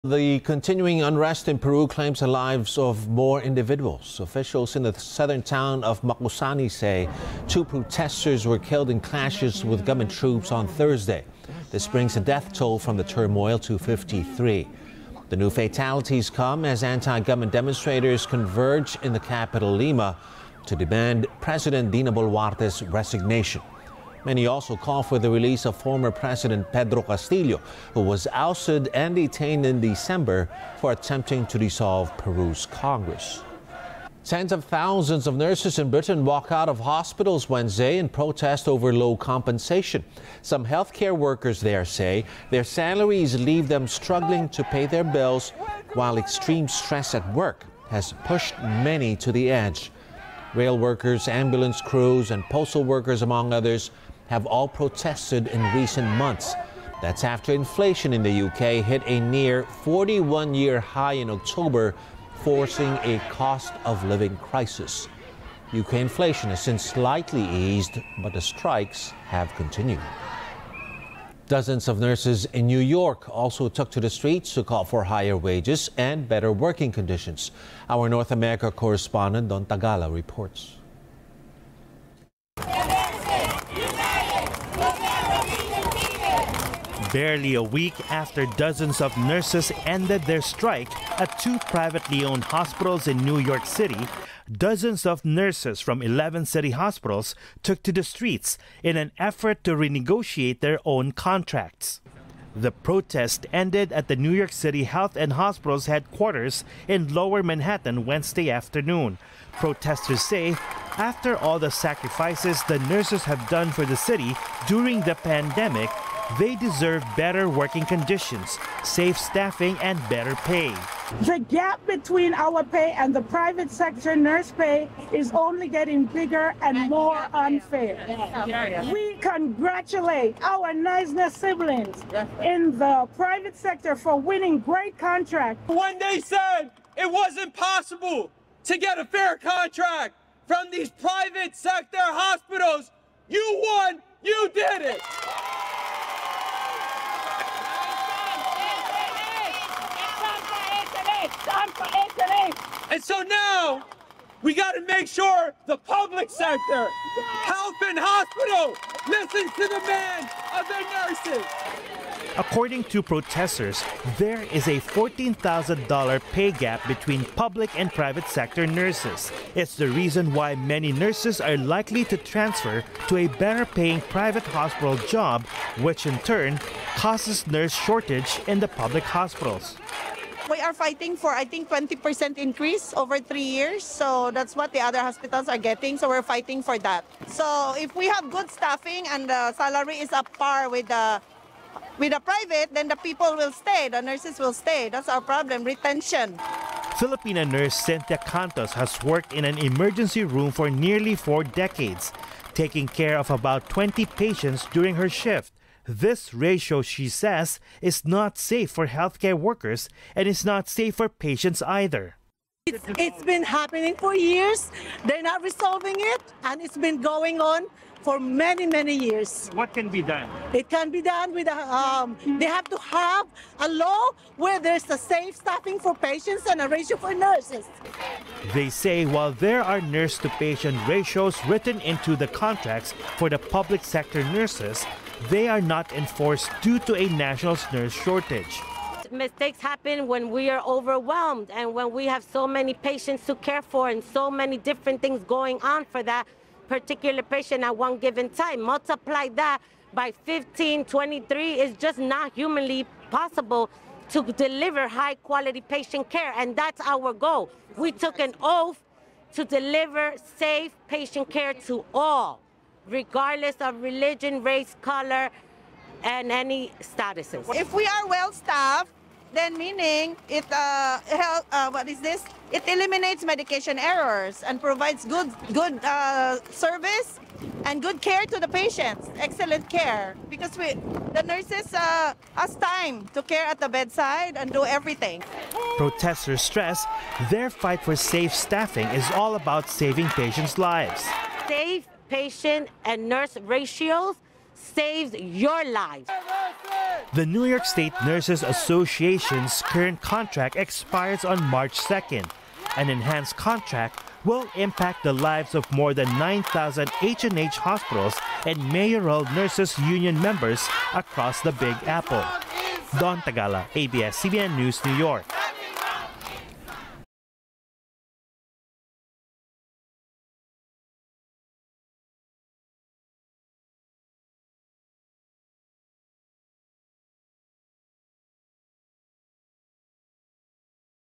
Two more protesters killed amid unrest in Peru; death toll now at 53 | ANC
Rundown: The continuing unrest in Peru claims the lives of two more individuals.